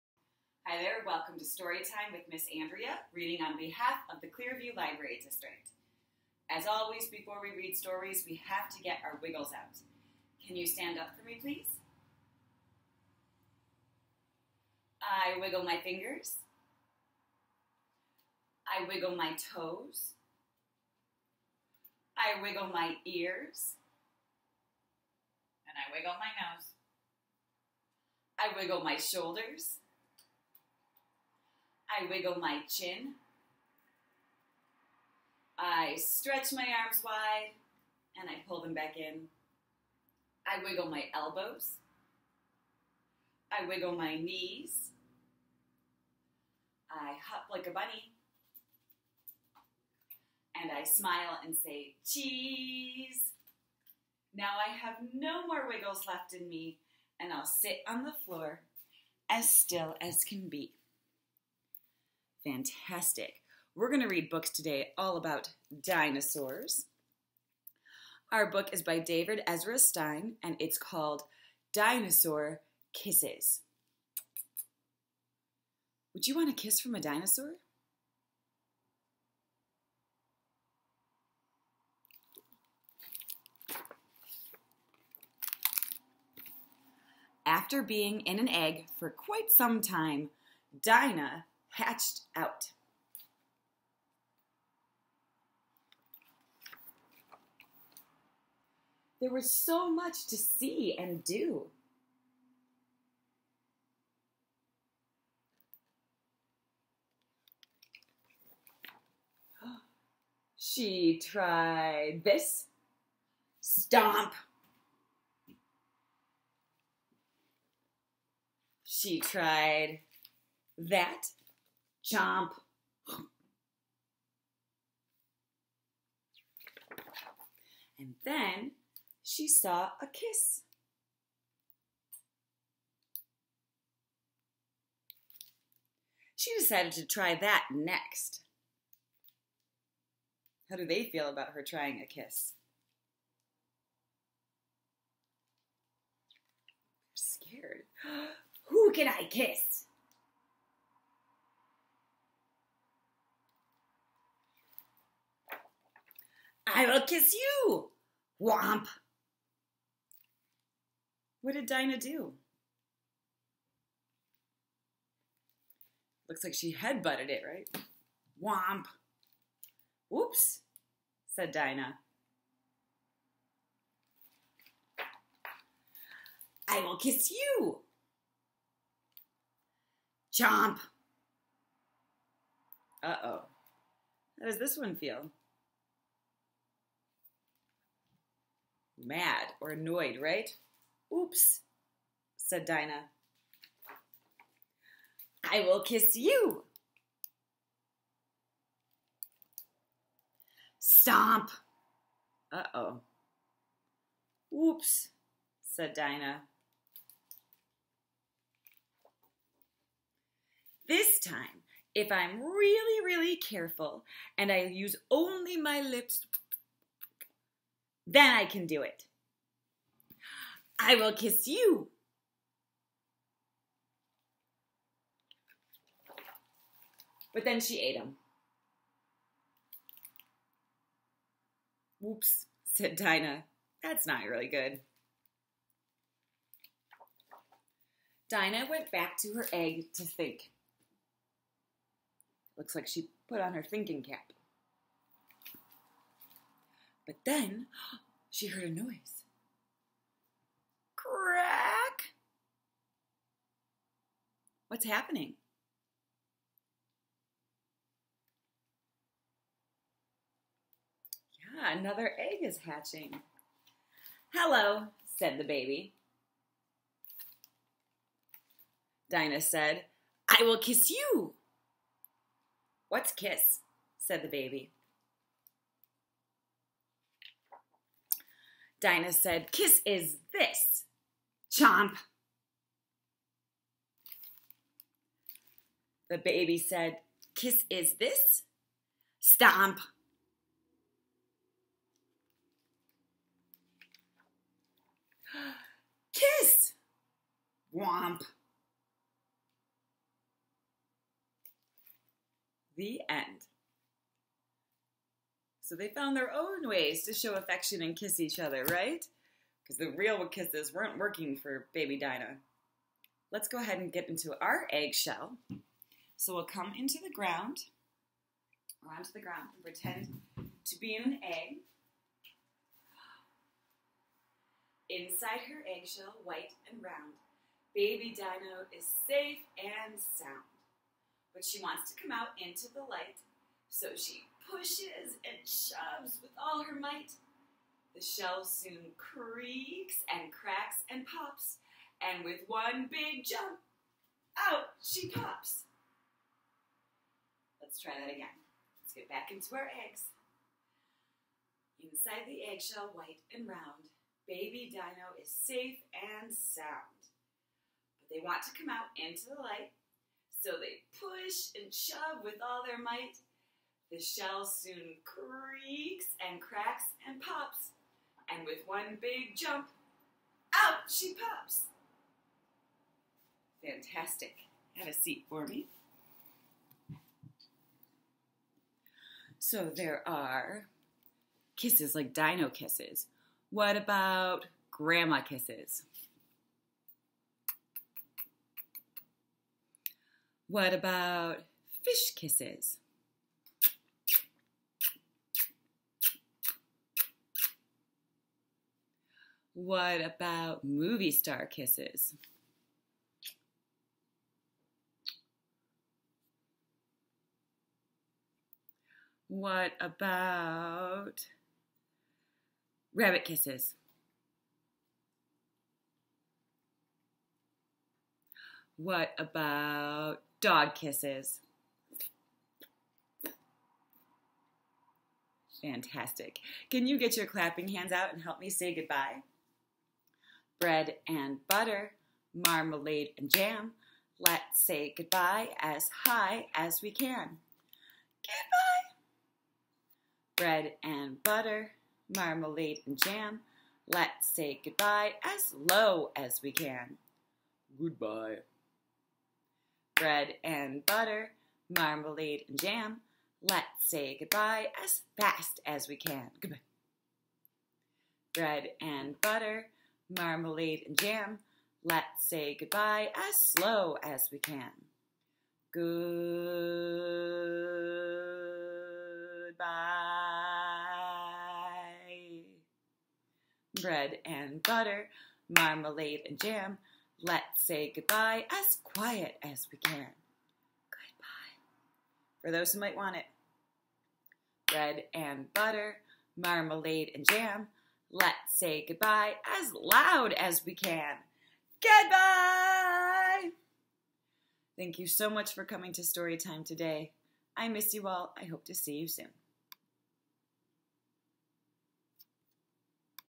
Storytime with Miss Andrea: 'Dinosaur Kisses' by David Ezra Stein
Join us or an active virtual story time featuring books, songs, music, and dancing with Miss Andrea!